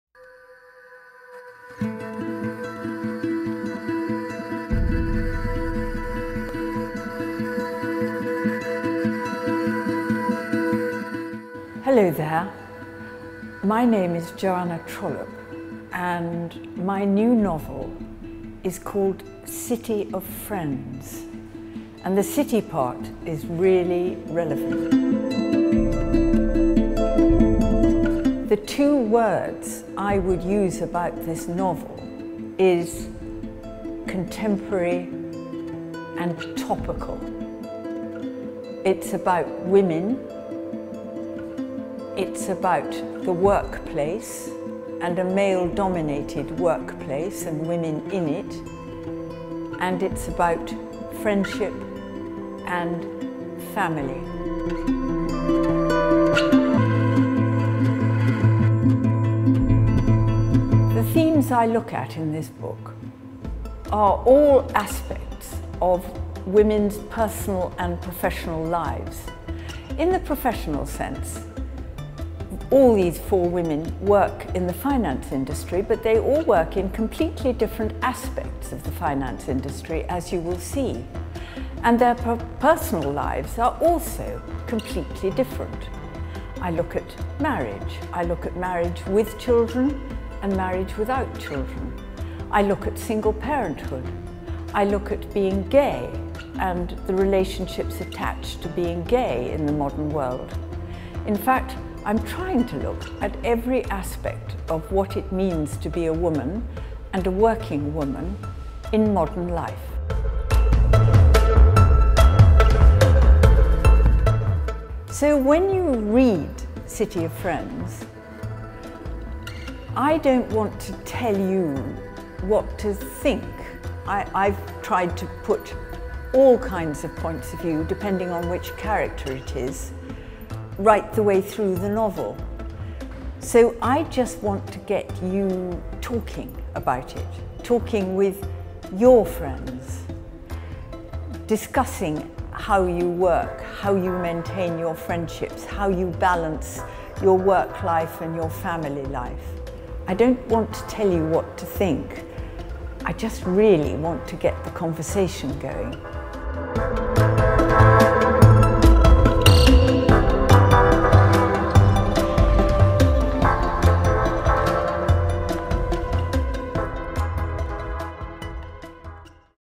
Joanna Trollope Interview: City of Friends
More about the book:

The day Stacey Grant loses her job feels like the last day of her life. Or at least, the only life she'd ever known. For who was she if not a City high-flyer, Senior Partner at one of the top private equity firms in London?

As Stacey starts to reconcile her old life with the new - one without professional achievements or meetings, but instead, long days at home with her dog and ailing mother, waiting for her successful husband to come home - she at least has The Girls to fall back on. Beth, Melissa and Gaby. The girls, now women, had been best friends from the early days of university right through their working lives, and for all the happiness and heartbreaks in between.

But these career women all have personal problems of their own, and when Stacey's redundancy forces a betrayal to emerge that was supposed to remain secret, their long cherished friendships will be pushed to their limits . . .